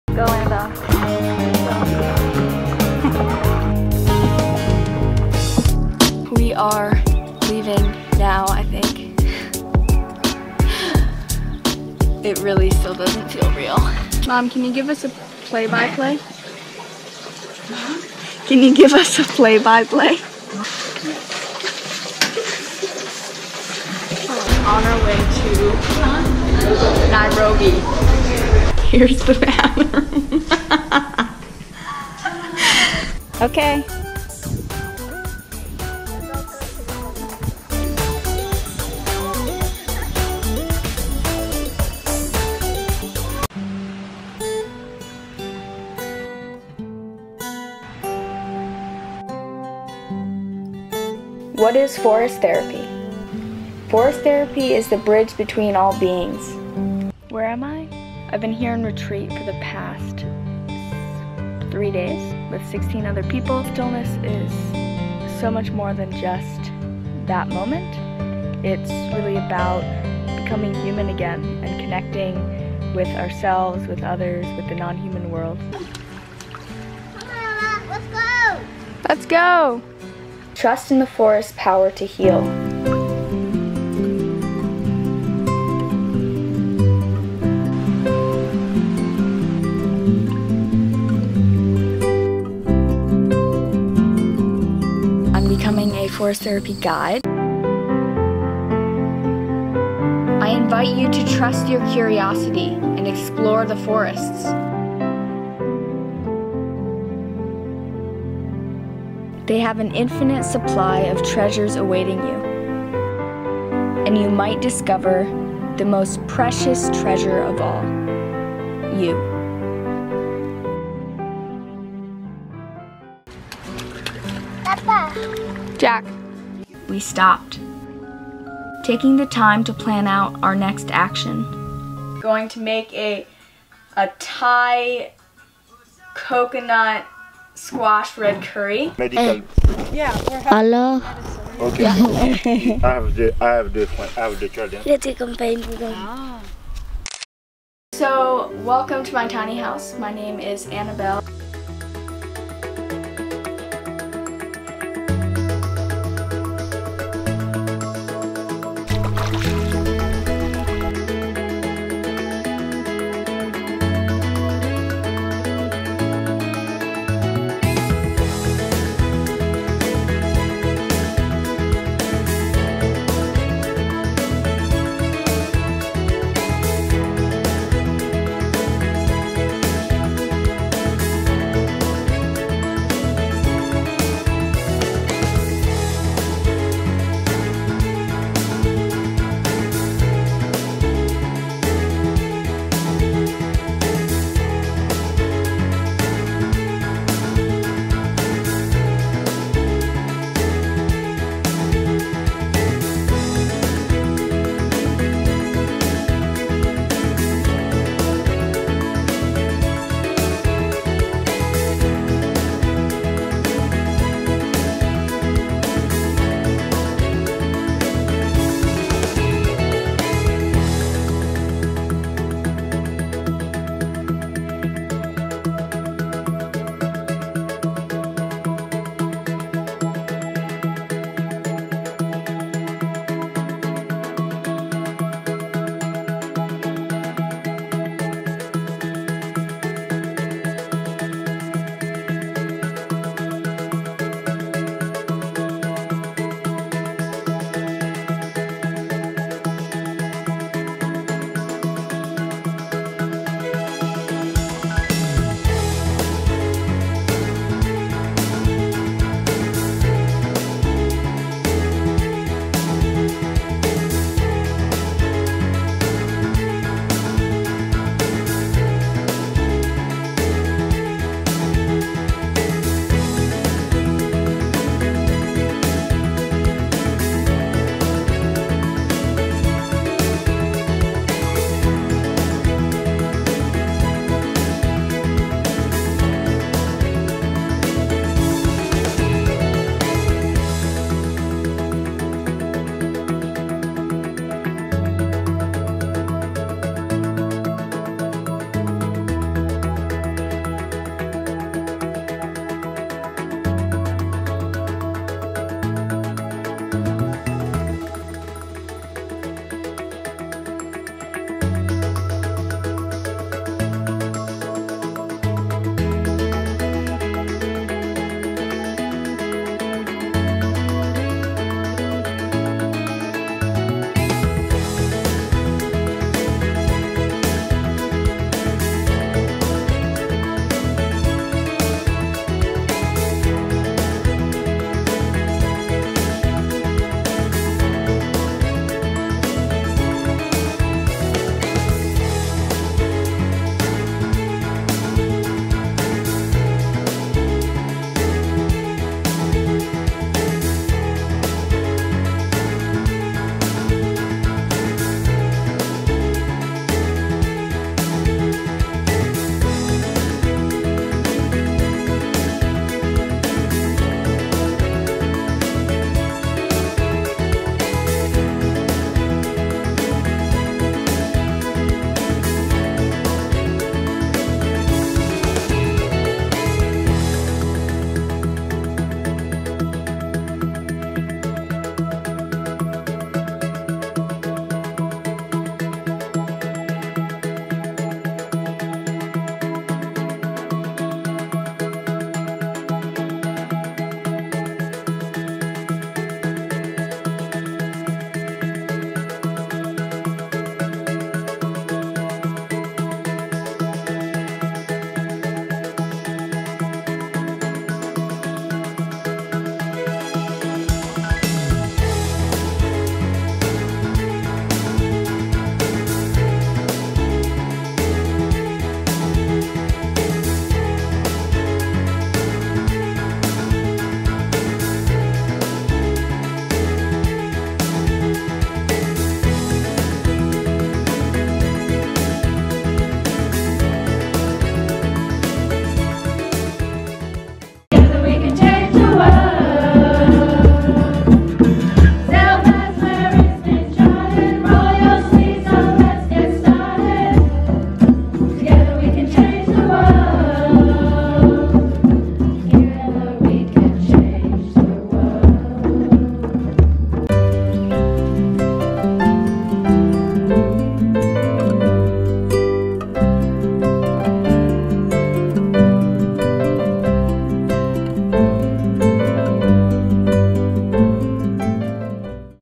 Highlights of 2017 || Year Review
This video is a collection of my favorite memories from 2017--traveling to Haiti, Madagascar, Puerto Rico, Florida & California as well as my tiny house, forest therapy and astrology adventure!!

TINY HOUSE EBOOK || Think Big. Build Tiny:

Send me mail:
Annabel O'Neill
POB 935

** VEGAN RESOURCES **